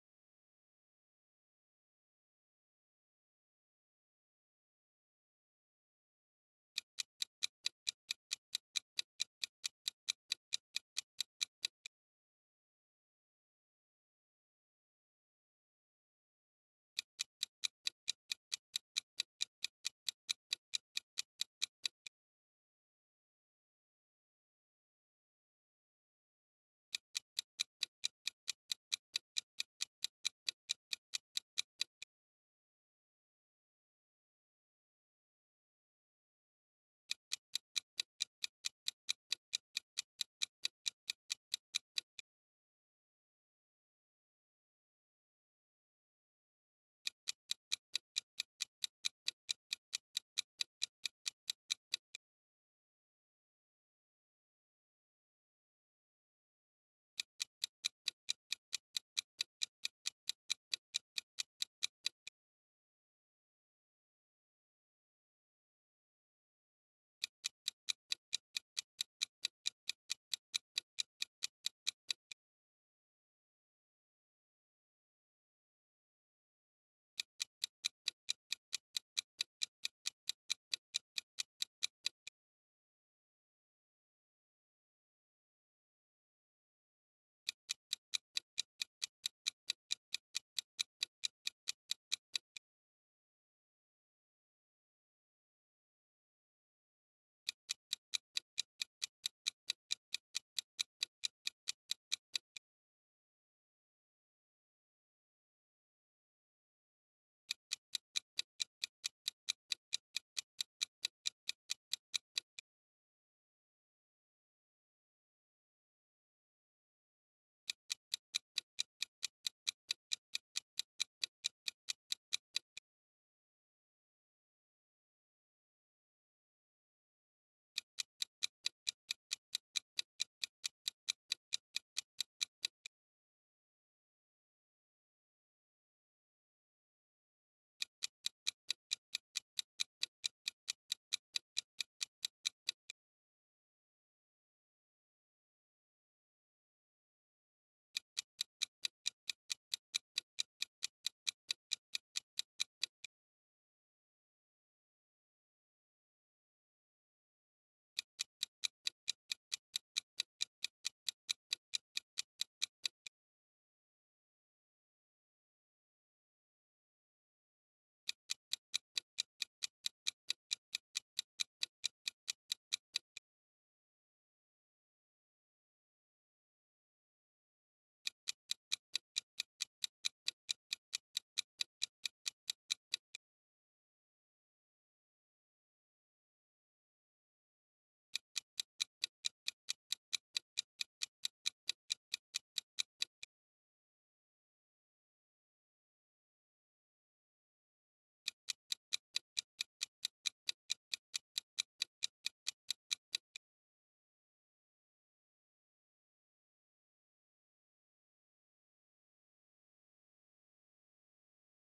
Guess The Food By Emoji | Food and Drink by Emoji Quiz
This is Guess The Food By Emoji | Food and Drink by Emoji Quiz
Can you Guess The Food by Emoji? This is a fun food and drink quiz you can play with your friends and family! Food game Emoji Challenge